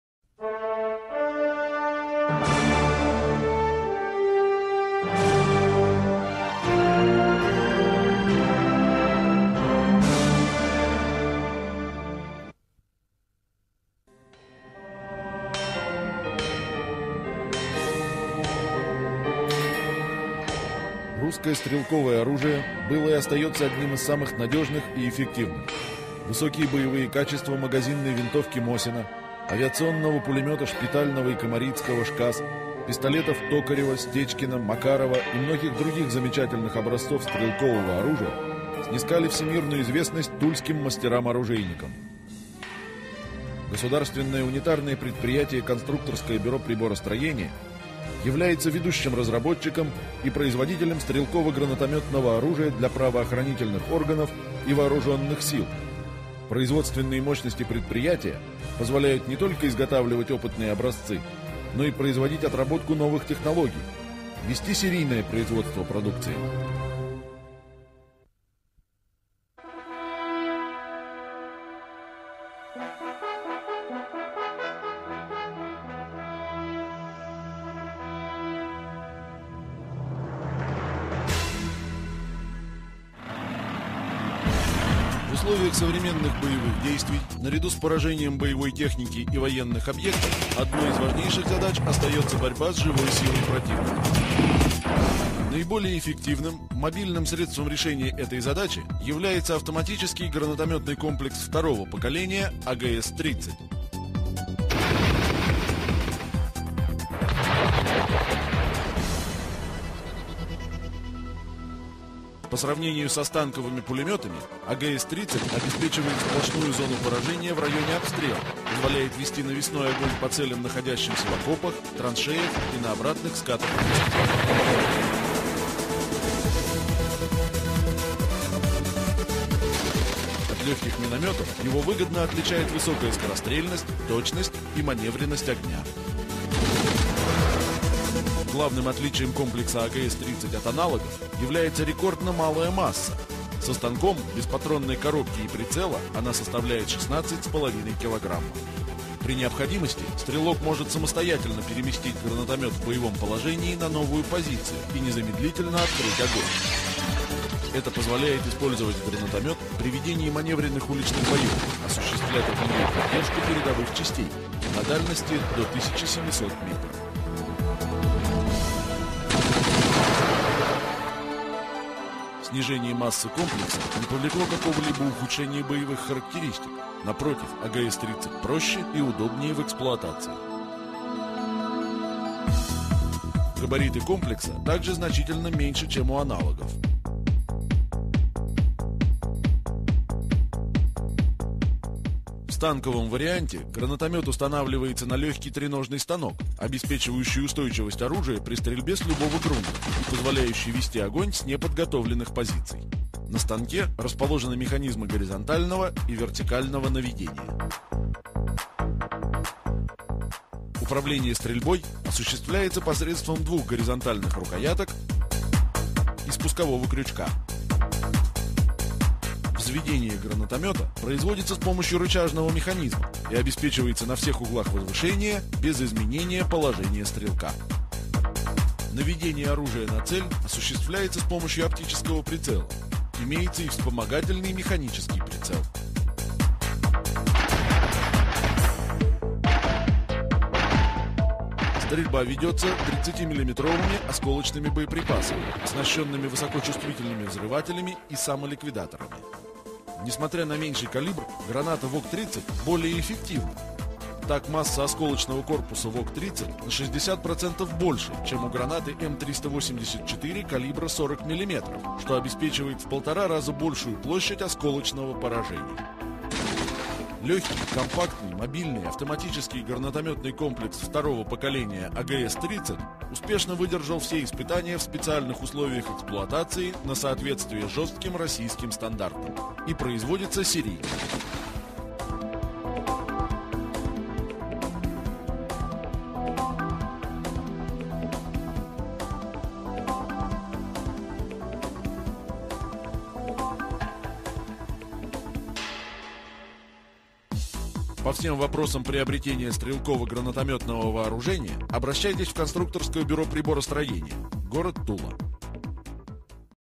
AGS-30 Russian Automatic Grenade Launcher
The AGS-30 is a Russian-designed automatic grenade launcher currently in production in the Russian Federation and in service with the Russian armed forces.